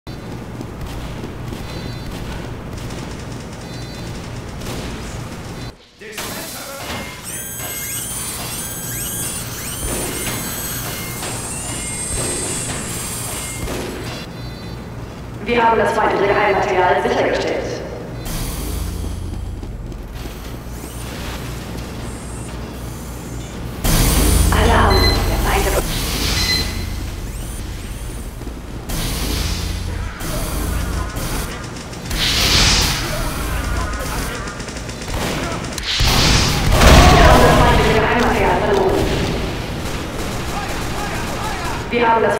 Engie Fail: Not My Finest Moment...
I choose the worst time to move my sentry. Not my finest moment, indeed...